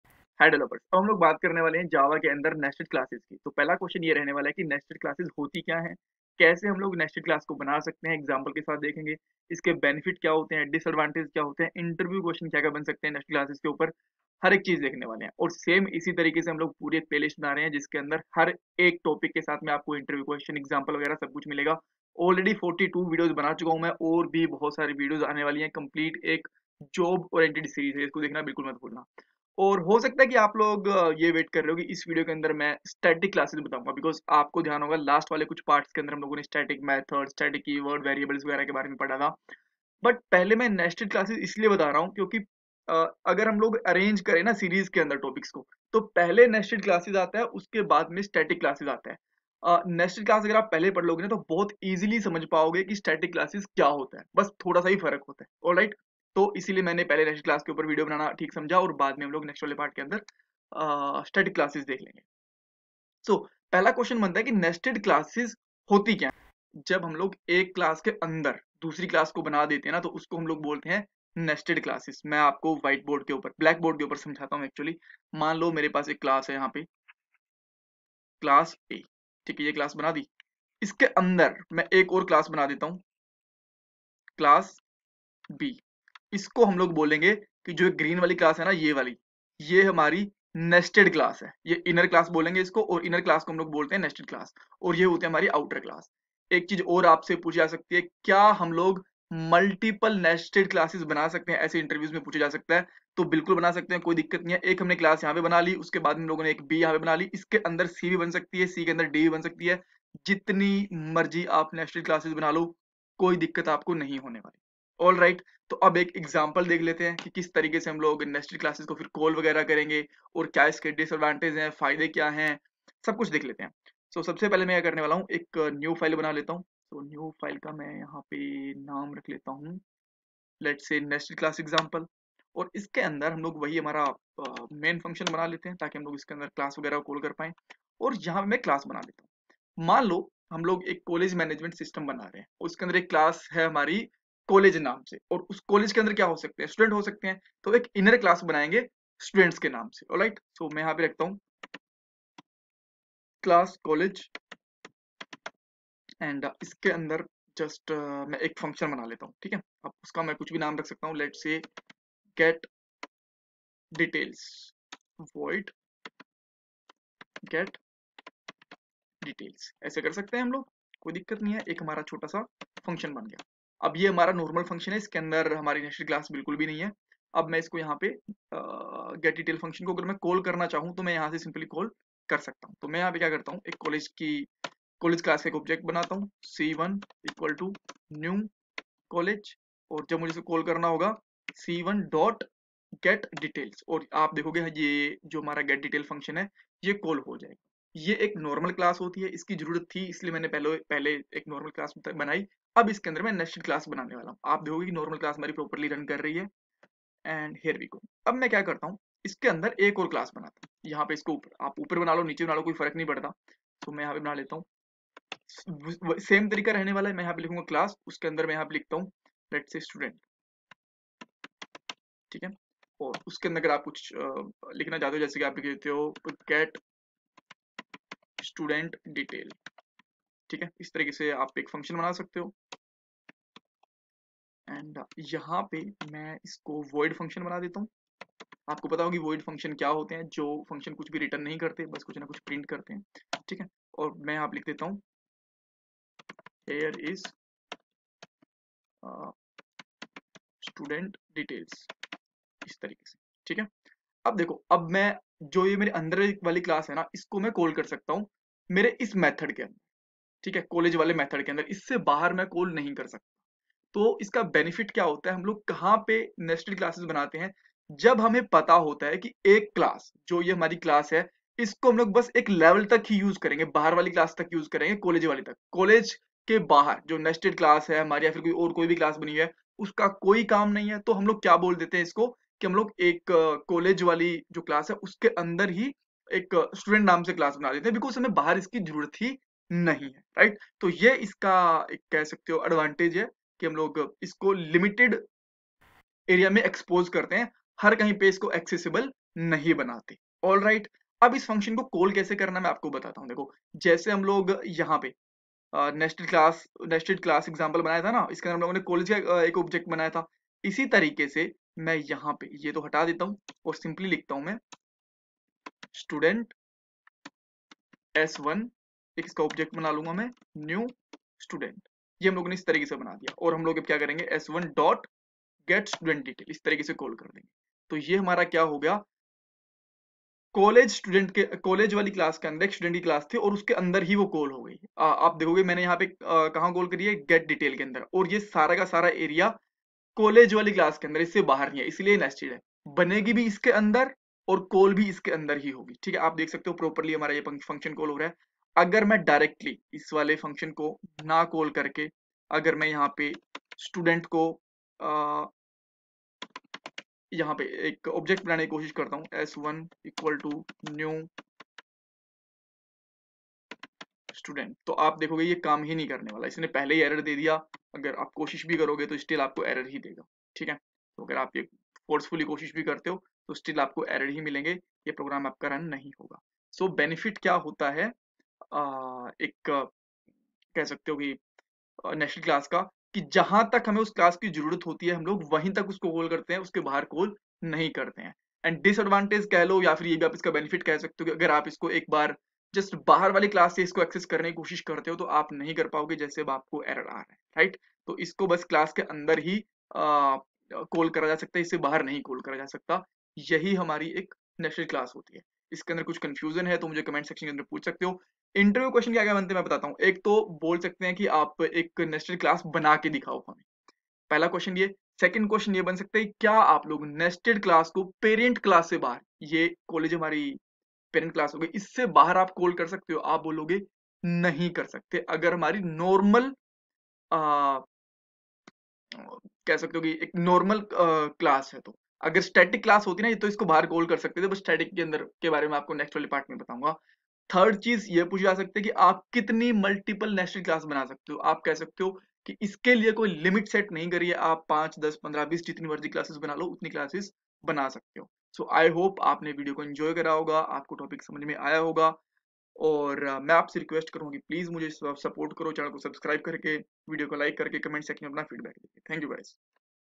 Java tutorial in Hindi for beginners #42 java Nested class in Hindi | Nested Classes
Here we learn about what is Nested classes variable Java in the Hindi language? This is a step-by-step guide to java in Hindi.

What is a Static keyword?
What is a static variable?
Examples of static Variable
Interview Questions
java tutorial for beginners step by step